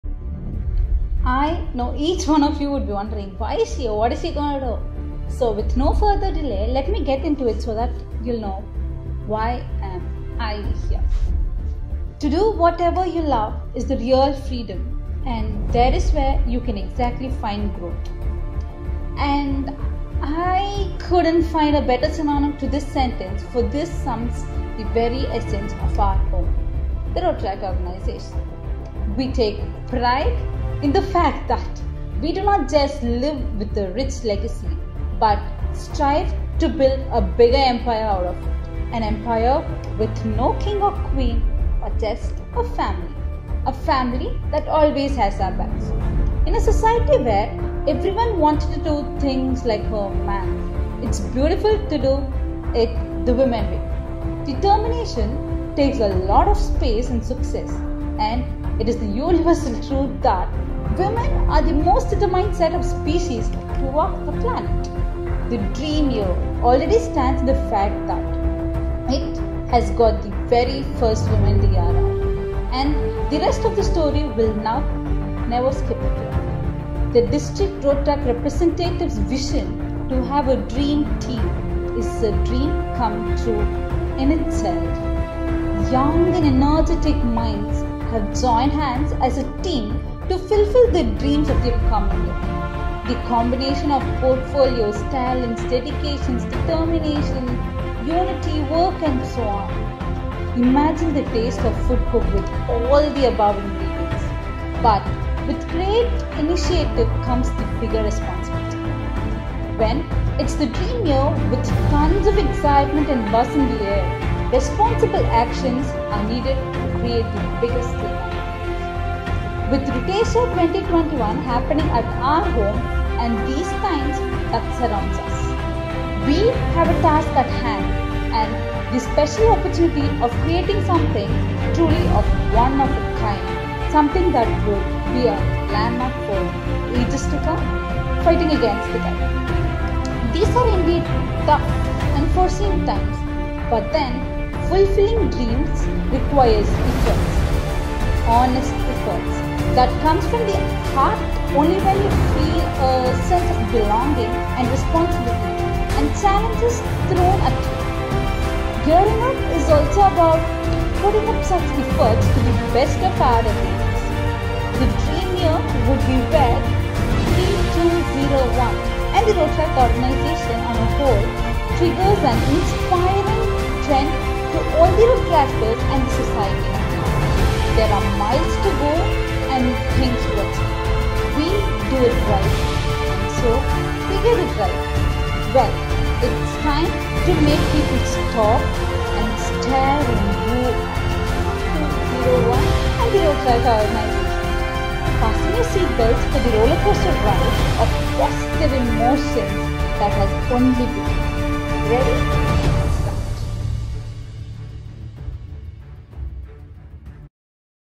Gearing up for Dream Year | Rotaract District 3201
Get to know what we're upto this Rotary year as our Special Correspondant Rtr. Varalakshmi explains on how we're gearing up for the Dream year